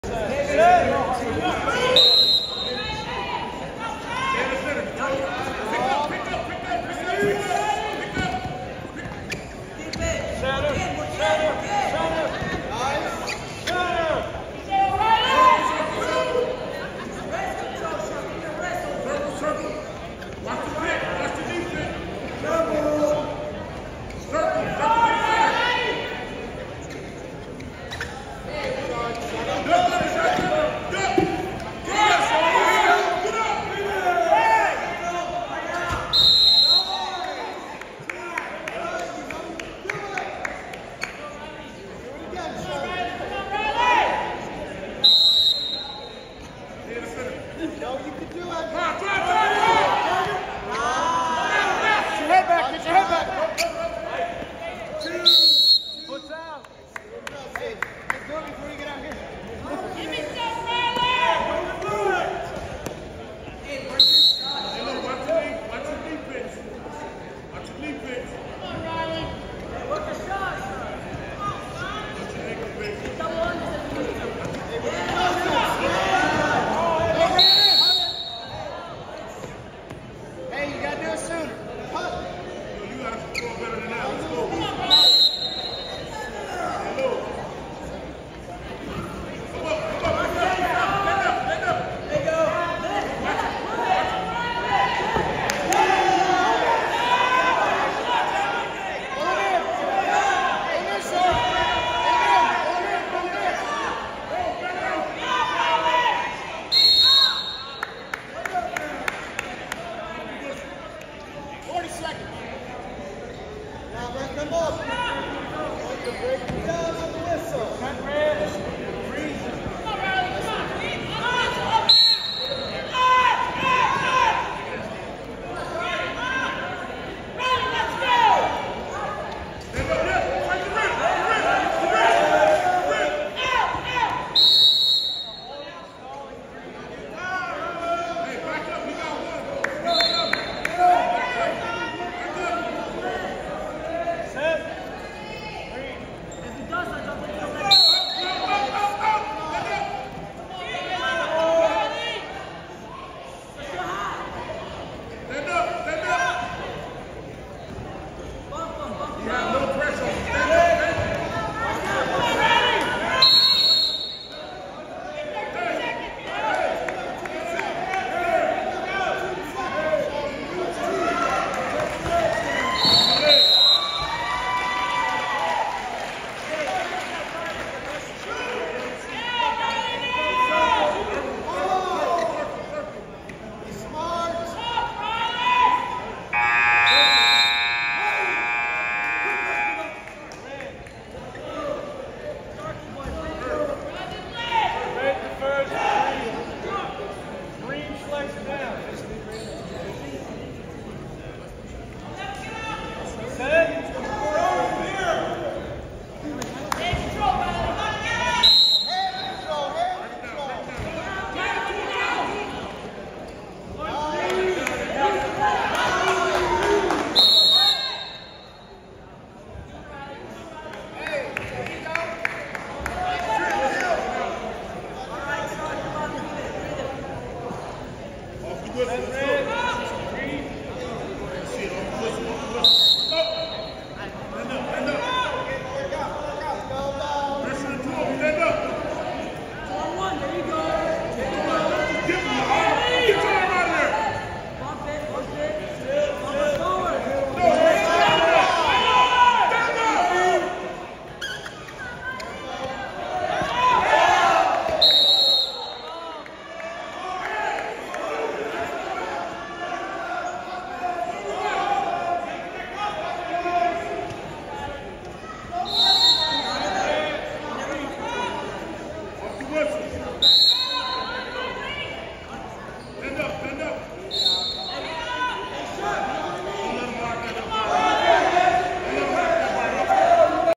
February 1, 2023
Various match clips from the MICAC Wrestling tournament at West Port High School Wednesday afternoon, February 1, 2023. Five local high schools competed in the meet, Dunnellon, Belleview, West Port, Forest and Lake Weir.